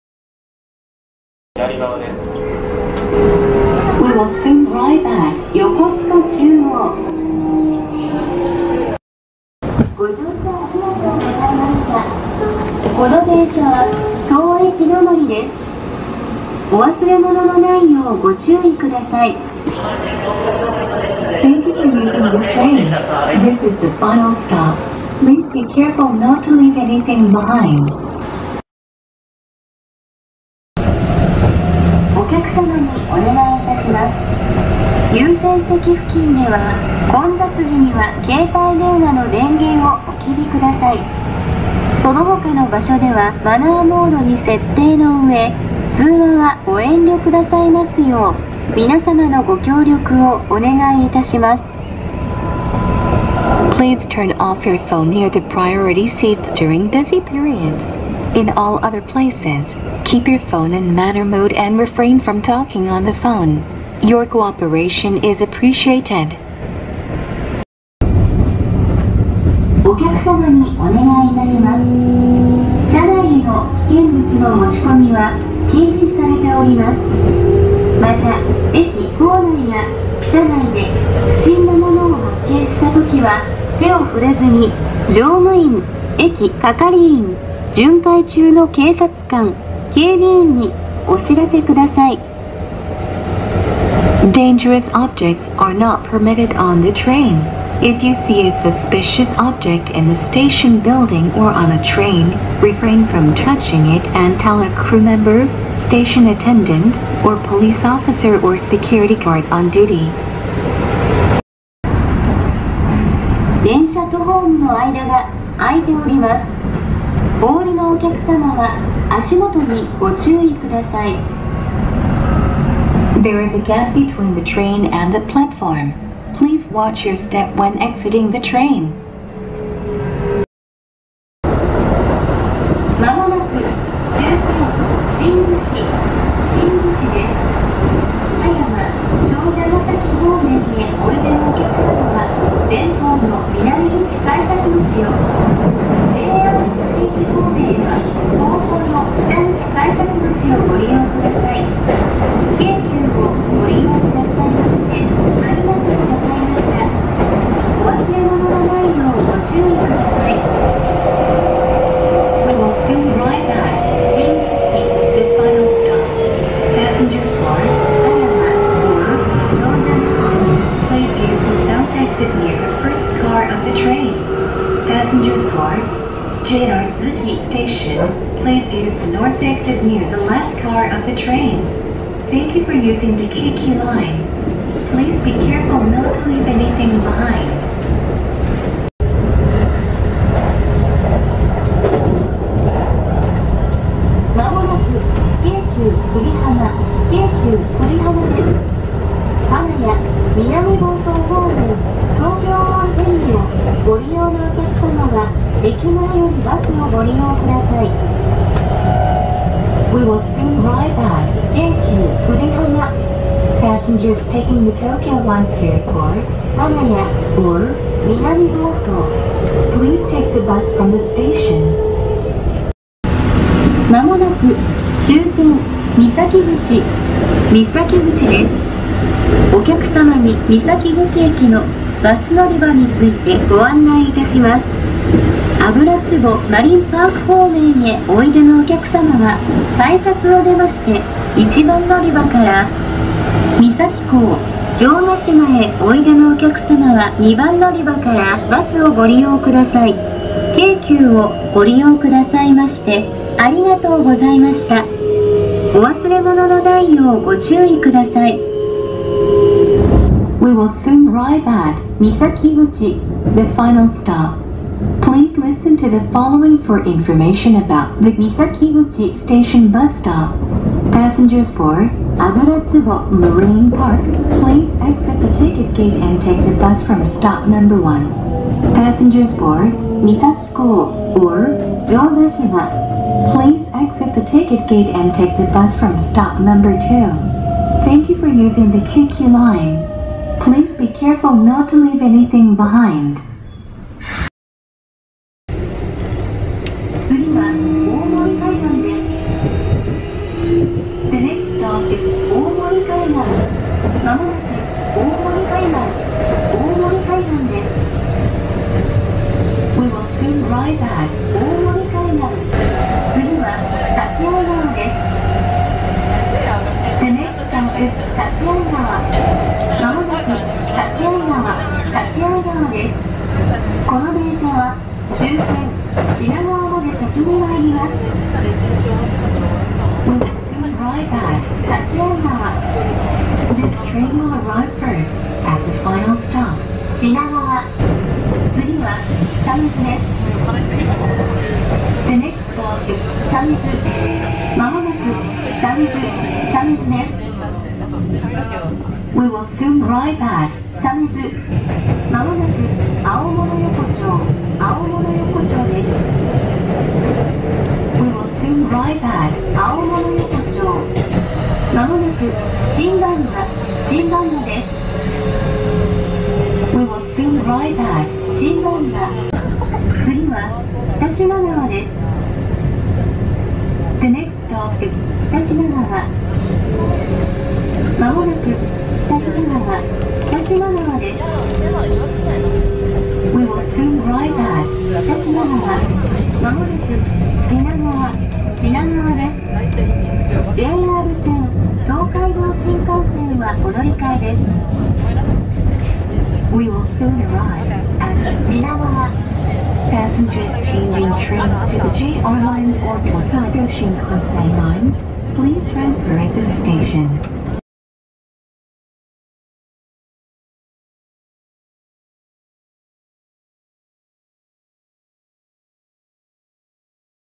【京急】旧タブレット自動放送集(字幕付き･一部･仮版2)
自分が収録したタブレット放送を1つの動画にまとめました。｢新逗子 バス｣が一番聞き取りづらいと思います。ご了承下さい。
一部の放送文面は、視聴者様から教えてもらいました。ありがとうございました。

【中央到着(英)】
We will soon arrive at, Yokosuka-chūō.

【終着】
ご乗車ありがとうございました。この電車は、当駅止まりです。お忘れもののないようご注意ください。
Thank you for using this train. This is the final stop. Please be careful not to leave anything behind.

【携帯マナー】
お客様に、お願い致します。優先席付近では、混雑時には、携帯電話の電源を、お切りください。そのほかの場所では、マナーモードに設定の上、通話は、ご遠慮くださいますよう、皆様のご協力を、お願い致します。
Please turn off your phone near the priority seats during busy periods. In all other places, keep your phone in manner mode and refrain from talking on the phone. Your cooperation is appreciated.

【不審物】
お客様に、お願い致します。車内への、危険物の持ち込みは、禁止されております。また、駅構内や、車内で、不審な物を発見したときは、手を触れずに、乗務員、駅係員、巡回中の警察官、警備員に、お知らせ下さい。
Dangerous objects are not permitted on the train.If you see a suspicious object in the station building or on a train, refrain from touching it, and tell a crew member, station attendant, or police officer or seculity gard on duty.

【曲線ホーム】
電車とホームの間が、空いております。お降りのお客様は、足元に、ご注意ください。
There is a gap between the train and the platform. Please watch your step when exiting the train.

【新逗子 バス】
まもなく、終点、新逗子、新逗子です。葉山、長者ヶ崎方面へ、おいでのお客様は、前方の、南口改札口を、JR逗子駅方面へは、後方の、北口改札口をご利用ください。京急を、ご利用くださいまして、ありがとうございました。お忘れもののないようご注意ください。
We will soon arrive at, Shinzushi, the final stop.Passengers for, Hayama, or, Chojagasaki, please use the south exit near the first car of the train. Passengers for, JR Zushi station, please use the north exit near the last car of the train.Thank you for using the Keikyu Line. Please be careful not to leave anything behind.

【久里浜 バス】
まもなく、京急久里浜、京急久里浜です。金谷、南房総方面、東京湾フェリーを、ご利用のお客様は、駅前よりバスをご利用ください。
We will soon arrive at, Keikyū Kurihama. Passengers taking the Tokyō-wan ferry for, Kanaya, or, Minamibōsō, please take the bus from the station.

【三崎口 バス】
まもなく、終点、三崎口、三崎口です。お客様に、三崎口駅の、バス乗り場について、ご案内いたします。油壺マリンパーク方面へ、おいでのお客様は、改札を出まして、1番乗り場から。三崎港、城ヶ島へ、おいでのお客様は、2番乗り場からバスをご利用ください。京急をご利用くださいまして、ありがとうございました。お忘れ物のないようご注意ください。
We will soon arrive at, Misakiguchi, the final stop. Please listen to the following for information about the Misakiguchi station, bus stop. Passengers for, Aburatsubo Marine Park. Please accent the ticket gate and take the bus from stop number one. Passengers for, misakikō, or, jōgashima. Please exit the ticket gate and take the bus from stop number two. Thank you for using the Keikyu Line. Please be careful not to leave anything behind.

4ページと5ページは、次駅案内と到着案内なので、各駅の文面は省略させて頂きます。

基本形
次は、○○です。The next stop is, ○○.
まもなく、○○、○○です。We will soon.arrive at, ○○.

【到着 立会川】
まもなく、立会川、立会川です。この電車は、終点、品川まで先に参ります。
We will soon arrive at, Tachiaigawa. This train will arrive first, at final stop, Shinagawa.

タブレット放送の内容は、固定コメントをご覧ください。
※字数制限のため、タブレット放送の内容を全て収めきれませんでした。